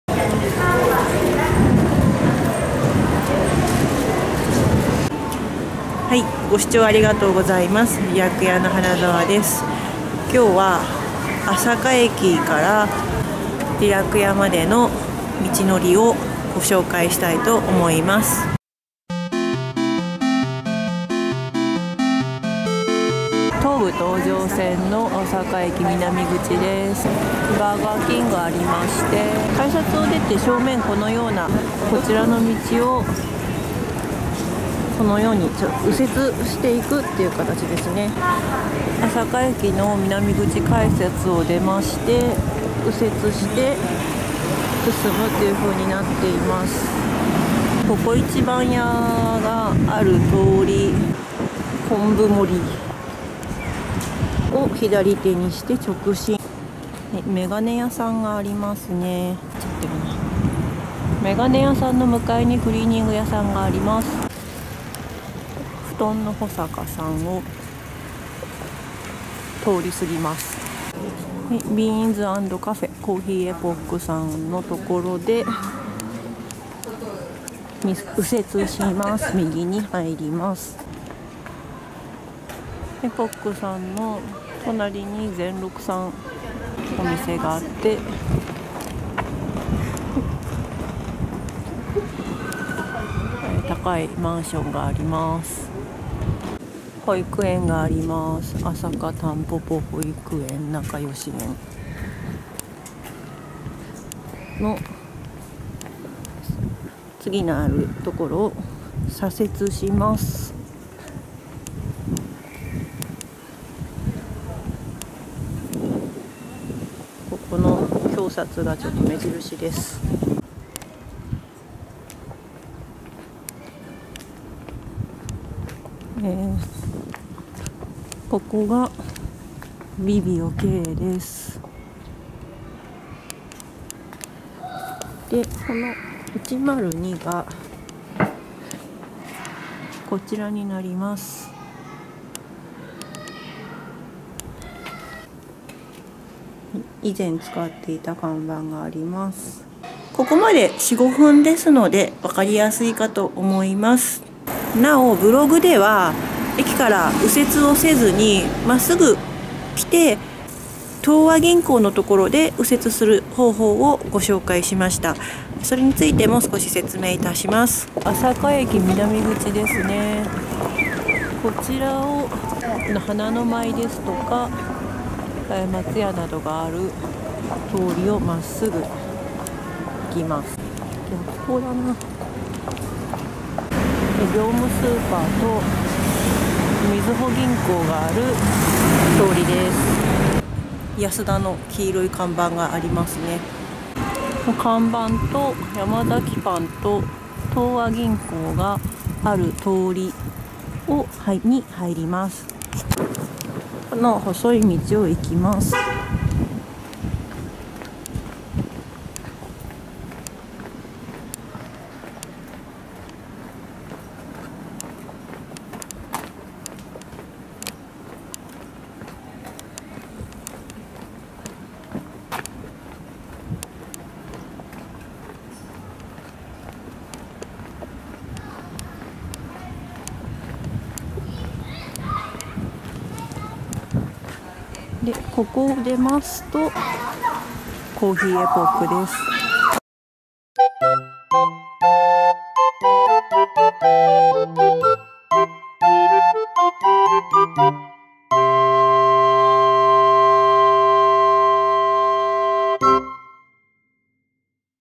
【道案内】りらく屋朝新店舗のアクセス２通り【朝霞駅南口から】
新しい店舗は、東武東上線 / 朝霞駅 徒歩4分の埼玉県朝霞市本町2丁目9-15 の１０２号室です。（完全予約制）